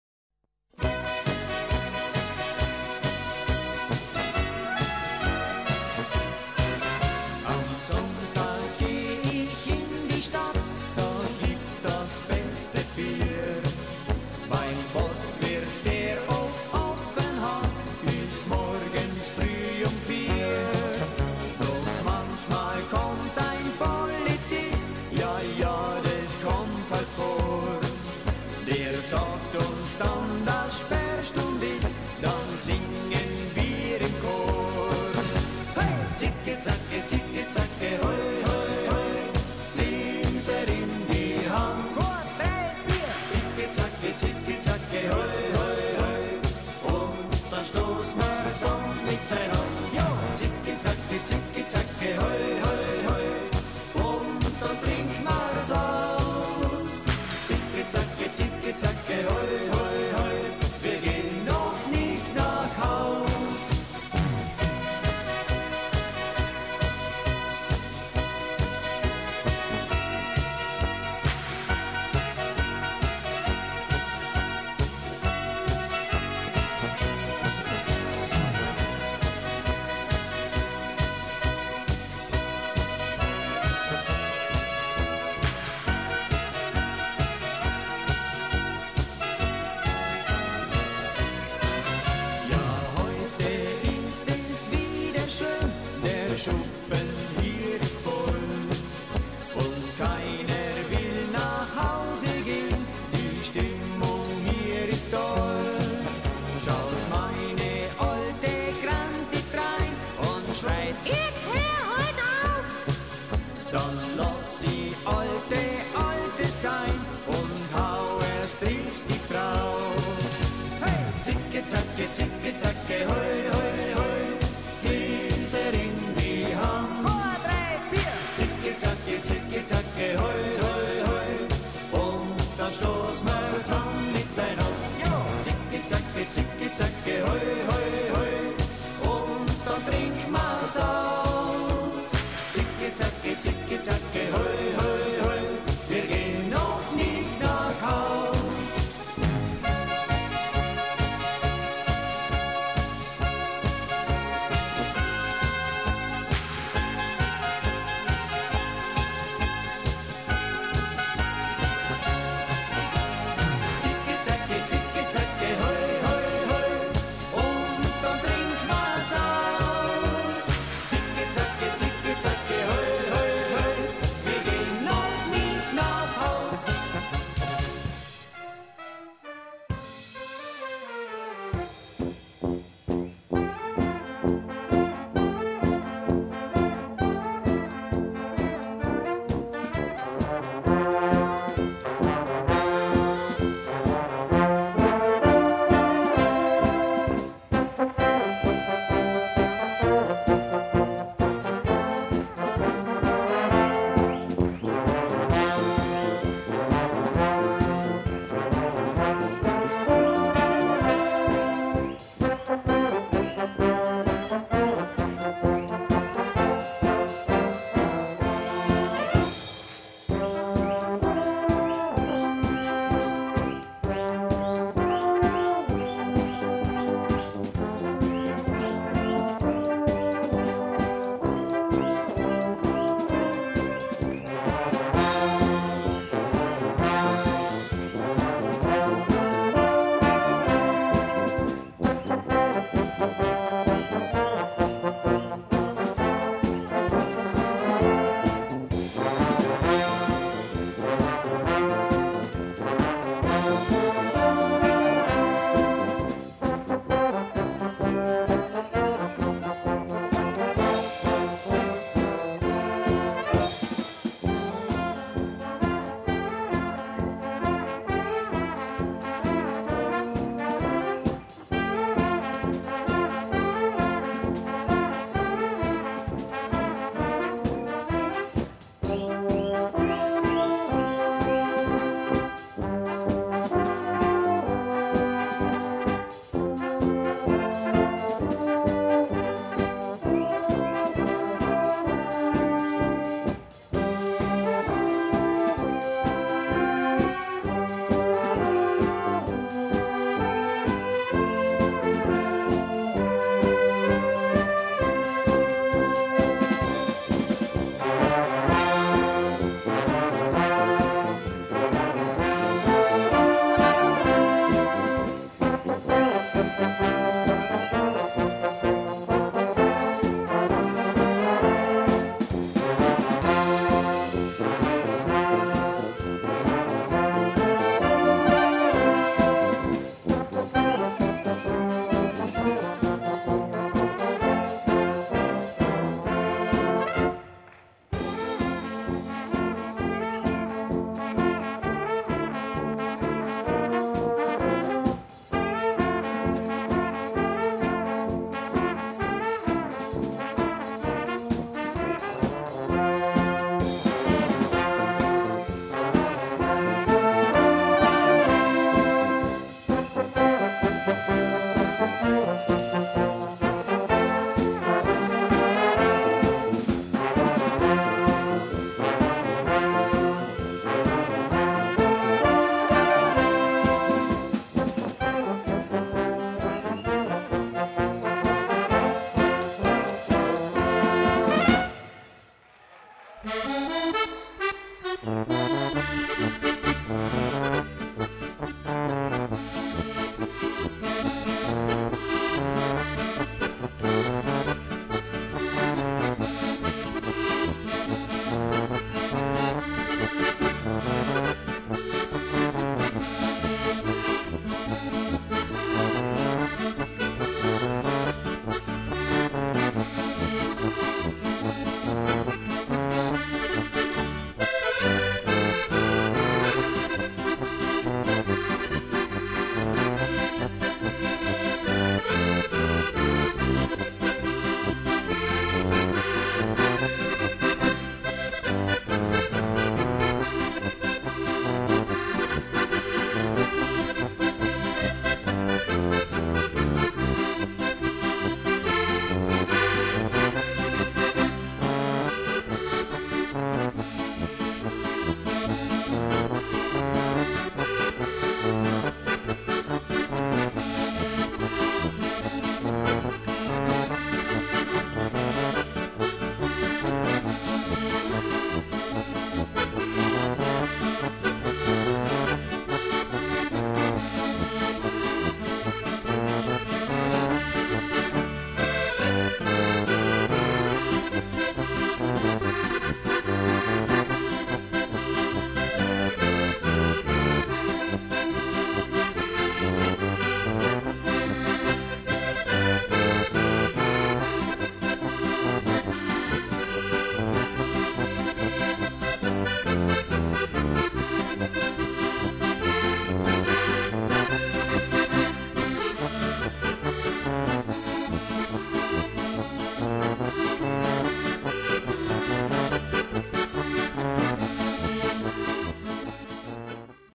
Presentación de jorgovan en la oktoberfest con clip Aleman
Mostrando nuestros Bailes y música hecho Clip